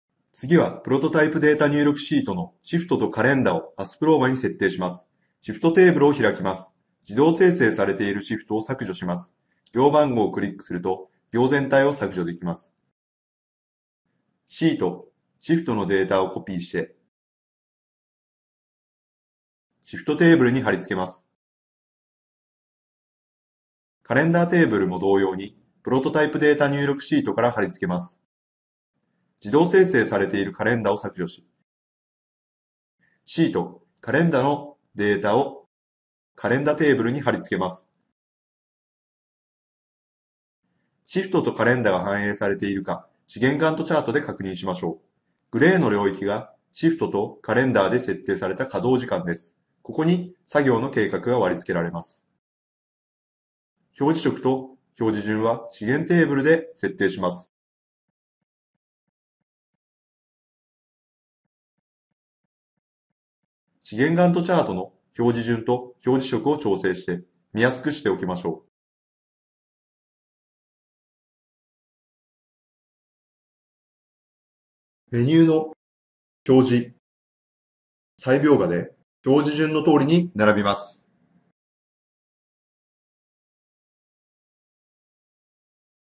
23.04 シフト、カレンダの設定と資源ガントチャート
次は、プロトタイプデータ入力シートのシフトとカレンダをAsprovaに設定します。

シフトテーブルを開きます。
自動生成されているシフトを削除します。
行番号をクリックすると行全体を削除できます。
シート　Shift　のデータをコピーして、シフトテーブルに貼り付けます。

カレンダテーブルも同様にプロトタイプデータ入力シートから貼り付けます。
自動生成されているカレンダを削除し、シート　Calendar　のデータをカレンダテーブルに貼り付けます。

シフトとカレンダが反映されているか、資源ガントチャートで確認しましょう。
グレーの領域がシフトとカレンダで設定された稼動時間です。
ここに作業の計画が割り付けられます。

資源ガントチャートの表示順と表示色を調整して、見やすくしておきましょう。
表示色と表示順は資源テーブルで設定します。

メニューの[表示]-[再描画]で、表示順の通りに並びます。